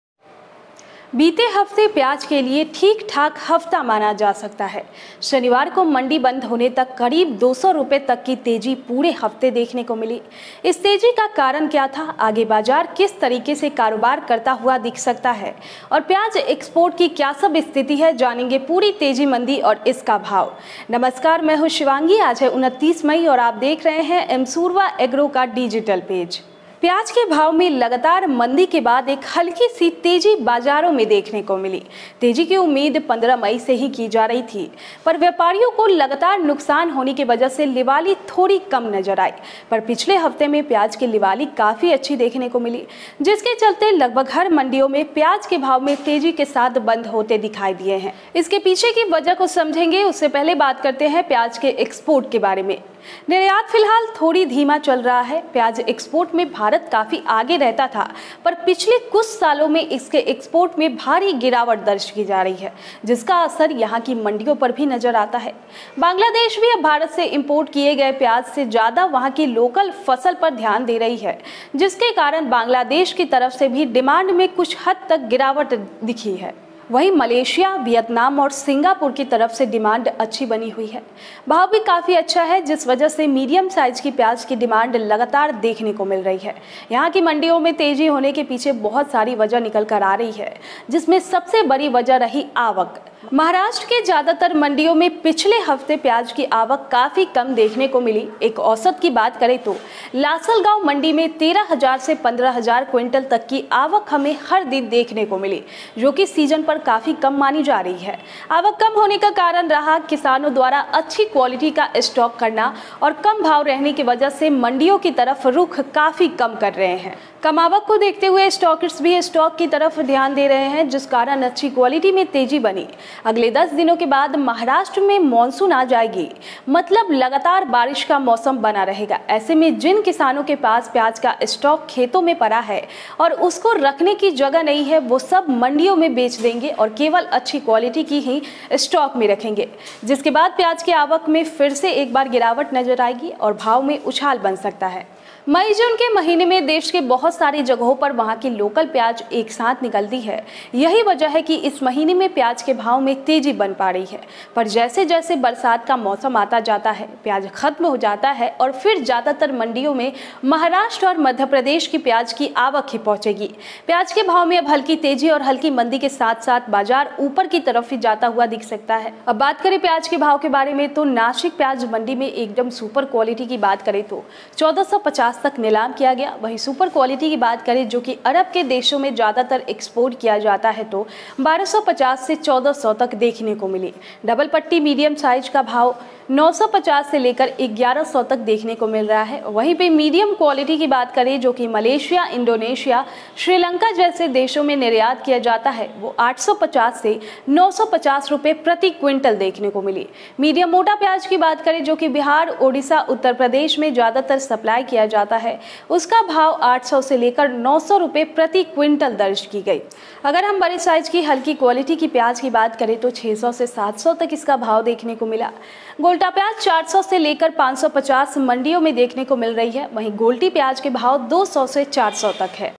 प्याज के भाव में तेजी आने की वजह | प्याज की आवक अब बहुत कम | प्याज में आगे भी रह सकती है तेजी
प्याज की तेजी मंदी रिपोर्ट | प्याज के भाव में तेजी बरक़रार | प्याज का भविष्य कैसा है